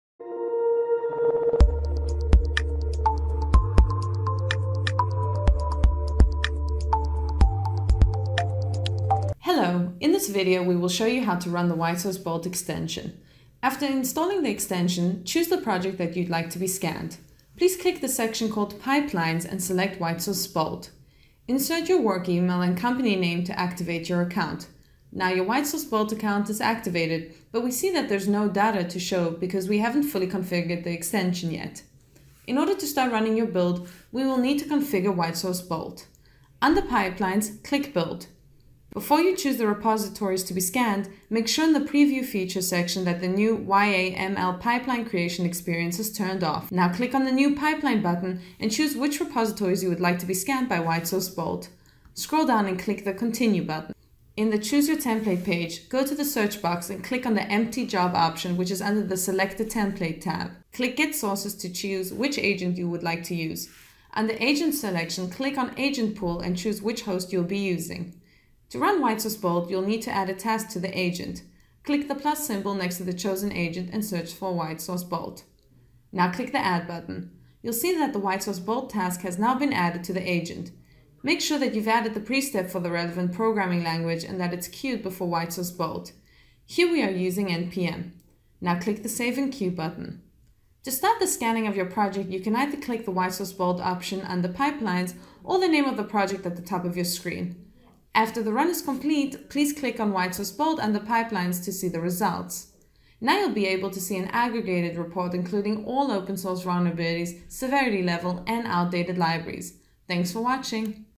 WhiteSource Bolt for Azure DevOps
Are you using Azure DevOps? Follow this short video and start using the WhiteSource Bolt to secure & manage your open source components automatically!